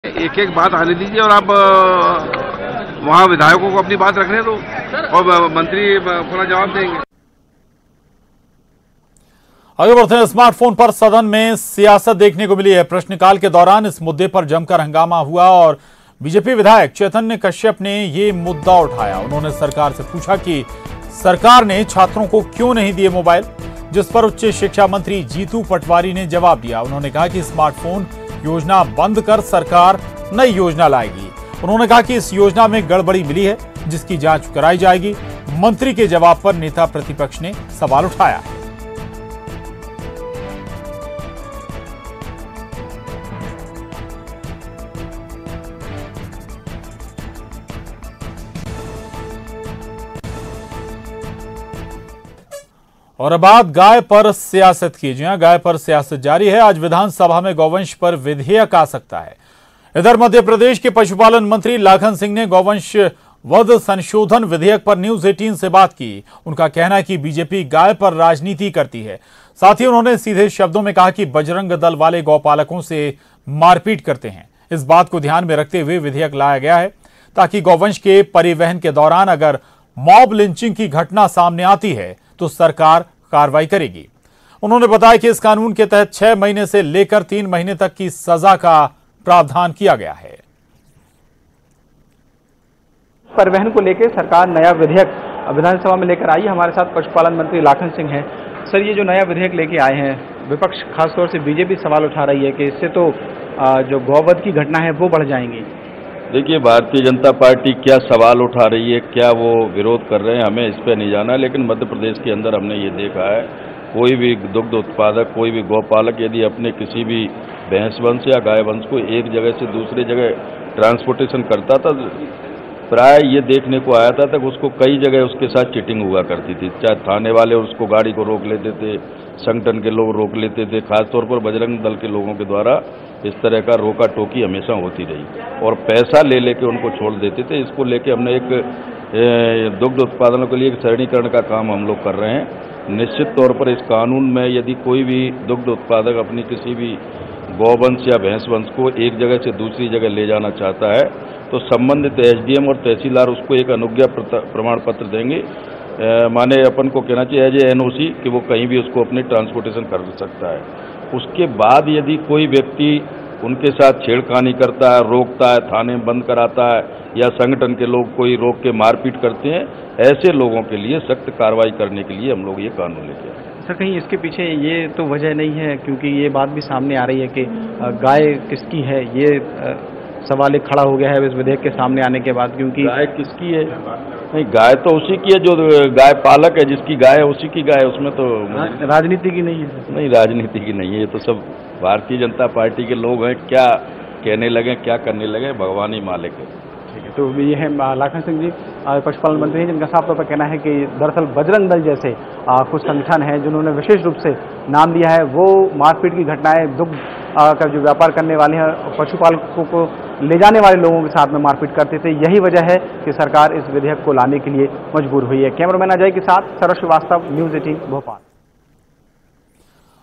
गोवंश पर सियासत तेज़, पशुपालन मंत्री Lakhan Singh बोले - "गौपालकों से मारपीट करते हैं बजरंग दल वाले"
गाय पर एक बार फिर सियासत तेज़ है। एम पी के पशुपालन मंत्री लाखन सिंह ने News18 से बातचीत में बताया कि बजरंग दल वाले गौपालकों से मारपीट करते हैं।

News18 MP Chhattisgarh Live TV | Watch Latest News In Hindi | हिंदी समाचार